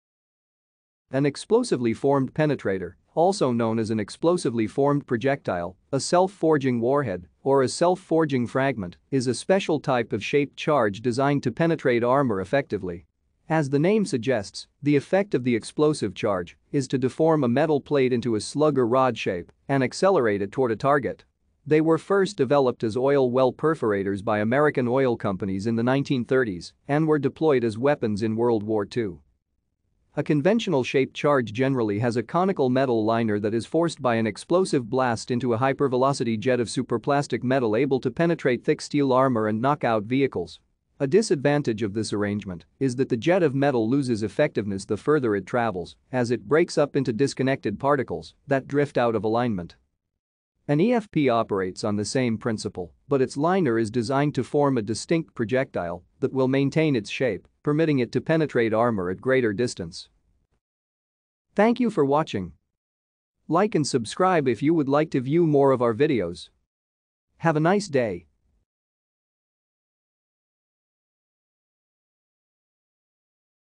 Explosively Formed Penetrator
An explosively formed penetrator, also known as an explosively formed projectile, a self-forging warhead, or a self-forging fragment, is a special type of shaped charge designed to penetrate armor effectively. As the name suggests, the effect of the explosive charge is to deform a metal plate into a slug or rod shape and accelerate it toward a target. They were first developed as oil well perforators by American oil companies in the 1930s, and were deployed as weapons in World War II.